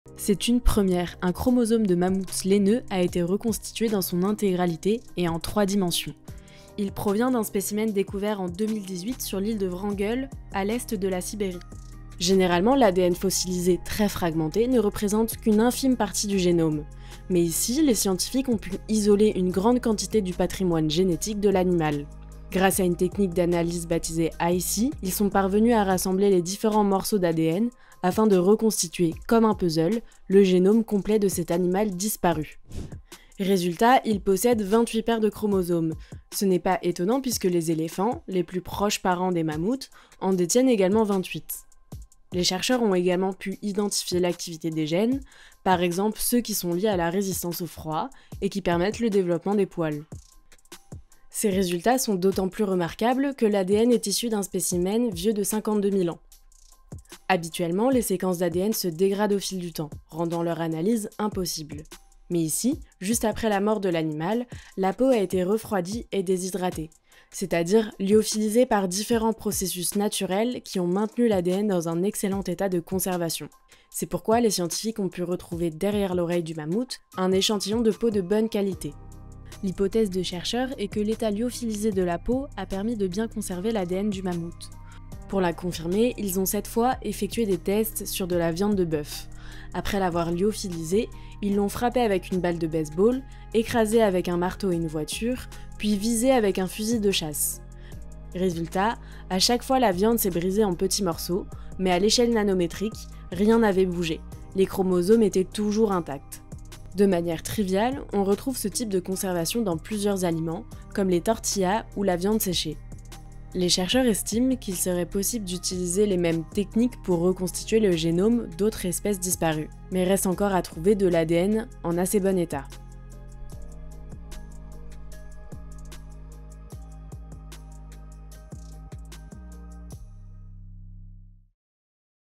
Le génome du mammouth laineux enfin reconstitué dans son intégralité en 3D | Actu de science
Le génome du mammouth laineux a été reconstitué dans son intégralité. Il provient d’un spécimen découvert en 2018 sur l’Île de Wrangel, dans l’est de la Sibérie. Grâce à une technique d’analyse baptisée Hi-C, les scientifiques sont parvenus à rassembler les différents morceaux d’ADN afin de reconstituer, comme un puzzle, le génome complet, de 28 chromosomes de cet animal disparu.